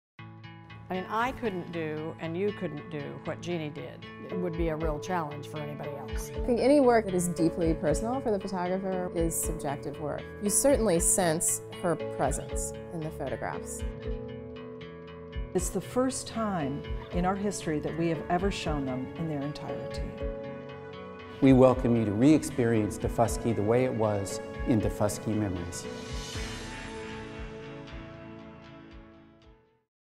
Daufuskie Memories
Open May 27- August 7 at the Columbia Museum of Art.

Through a series of over 60 photographs taken from 1977–1982, Jeanne Moutoussamy-Ashe vividly and poignantly captured African-American life on Daufuskie Island, South Carolina. In that time, Daufuskie Island stood alone as the last South Carolina sea island untouched by the outside world, serving as a home to residents whose Gullah lifestyle, traditions, and language were preserved because of their isolation. Recognizing this unique culture and the need to capture this way of life, Moutoussamy-Ashe used her camera to chronicle the life of the island.